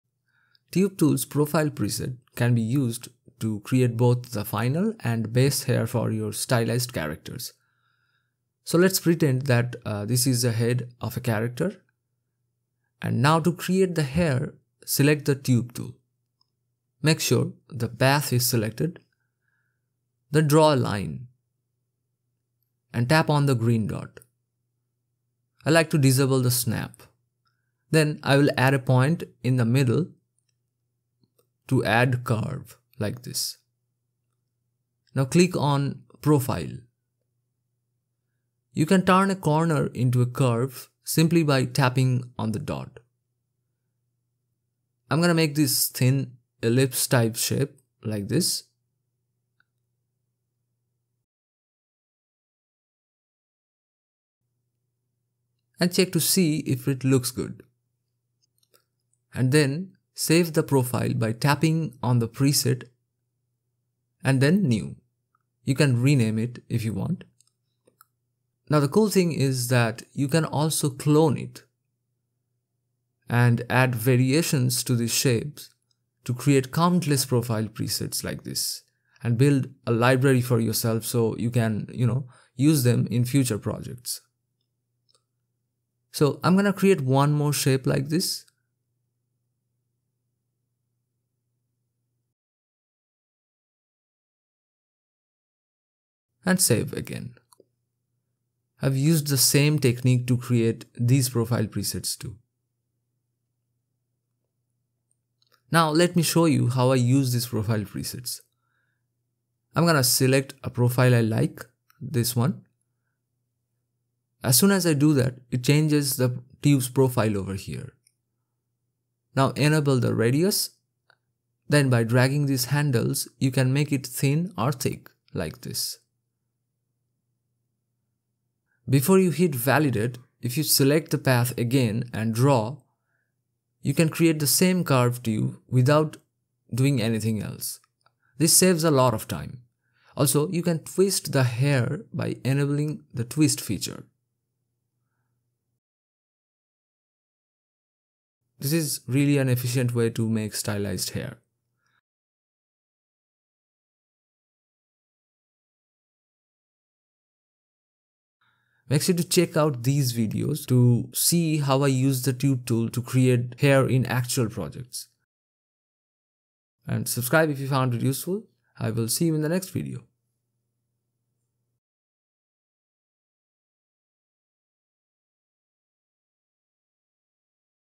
Create Stylized Hair | Nomad Sculpt Tutorial
In this Nomad Sculpt Tutorial I will teach how to create hair for stylized characters with the help of the Tube Tool.

Other Recommended Tutorials

Chapters

0:00 Intro
0:08 How To Create
01:44 How To Use